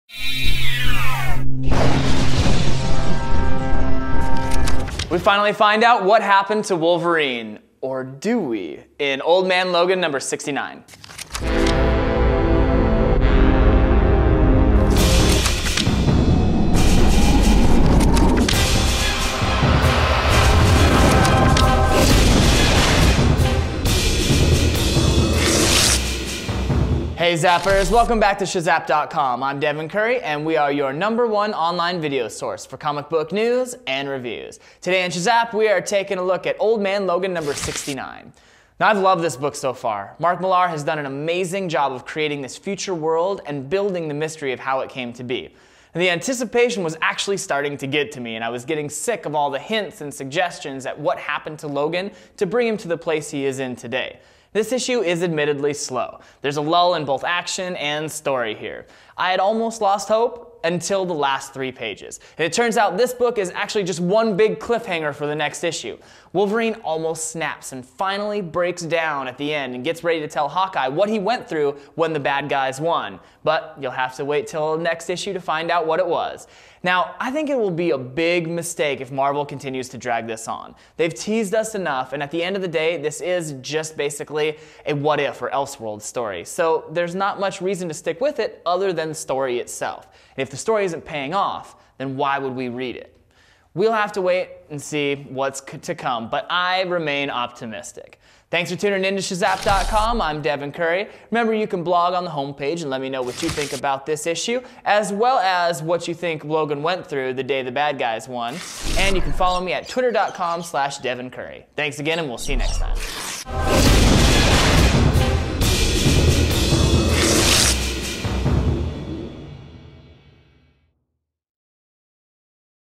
Old Man Logan #69 - Comic Review - Shazap.com
Your online video source for comic book news and reviews.

I&rsquo;ve loved this book so far, Mark Millar has done an amazing job of creating this future world and building the mystery of how it came to be.  The anticipation was getting to me and I was getting sick of the hints and suggestions at what had happened to Logan to bring him to the place he is today.  This issue is admittedly slow.  There is a lul in both action and story.  I had almost lost hope until the last three pages.  This issue is just one big cliff hanger.   Wolverine almost snaps and finally breaks down and gets ready to tell Hawkeye what he went through when the bad guys won.  Now it will be a big mistake if Marvel continues to drag this on.  They have teased us enough and at the end of the day this is just and basic what if or else world story so there is not much reason to stick with it other than the story itself.  If the story isn&rsquo;t paying off then there is no reason to keep reading.  We&rsquo;ll have to wait and wait and see what is to come but I remain optimistic.  Distributed by Tubemogul.